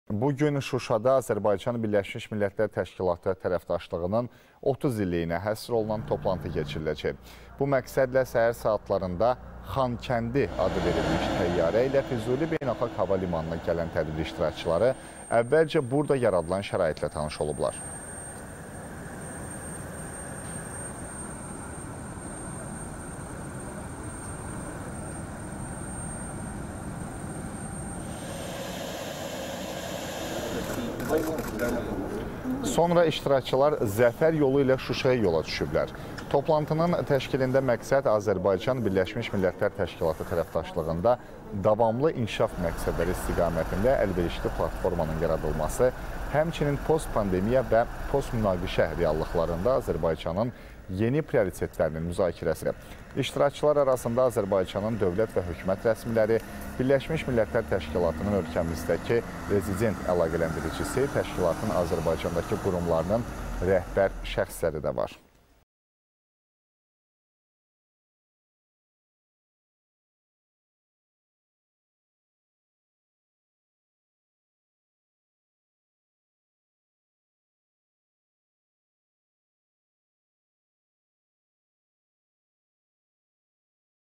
Tədbir iştirakçıları “Xankəndi” adı verilmiş təyyarə ilə Füzuli Beynəlxalq Hava Limanına gəldi
Bu gün Şuşada Azərbaycan-BMT tərəfdaşlığının 30 illiyinə həsr olunan toplantı keçiriləcək. Bu məqsədlə səhər saatlarında “Xankəndi” adı verilmiş təyyarə ilə Füzuli Beynəlxalq Hava Limanına gələn tədbir iştirakçıları əvvəlcə burada yaradılan şəraitlə tanış olublar. Sonra iştirakçılar Zəfər yolu ilə Şuşaya yola düşüblər. Toplantının təşkilində məqsəd Azərbaycan-BMT tərəfdaşlığında Davamlı İnkişaf Məqsədləri istiqamətində əlverişli platformanın yaradılması, həmçinin postpandemiya və postmünaqişə reallıqlarında Azərbaycanın yeni prioritetlərinin müzakirəsidir. İştirakçılar arasında Azərbaycanın dövlət və hökumət rəsmiləri, BMT-nin ölkədəmizki rezident əlaqələndiricisi, təşkilatın Azərbaycandakı qurumlarının rəhbər şəxsləri də var.